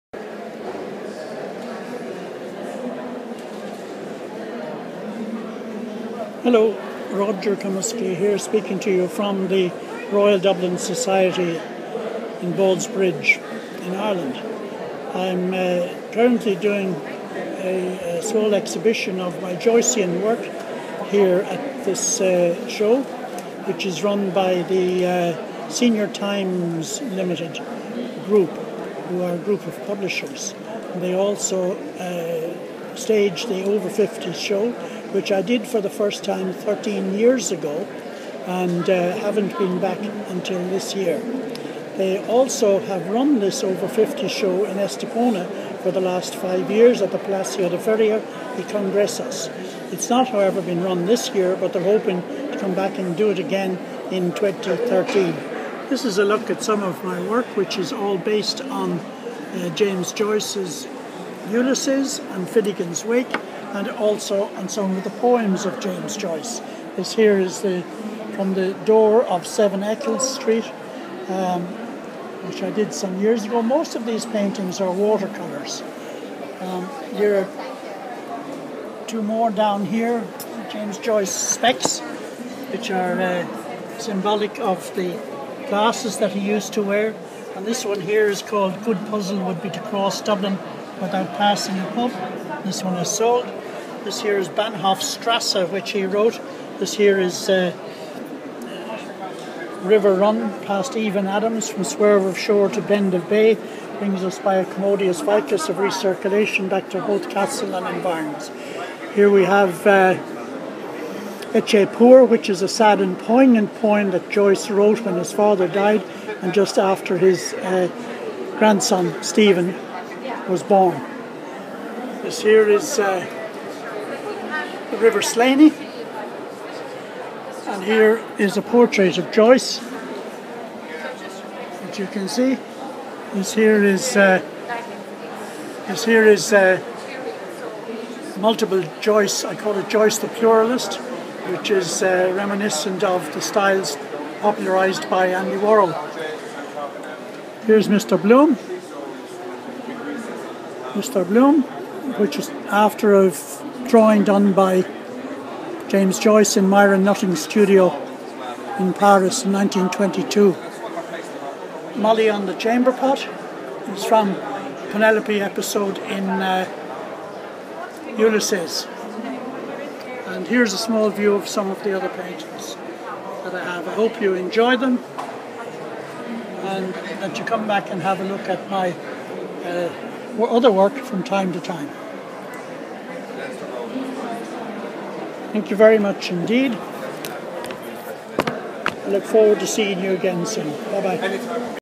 Roger at the RDS, DUBLIN, October 2012.
My booth at the SeniorTimes ltd Over50s Show at the Royal Dublin Society.

With thanks to John Low and Des Duggan.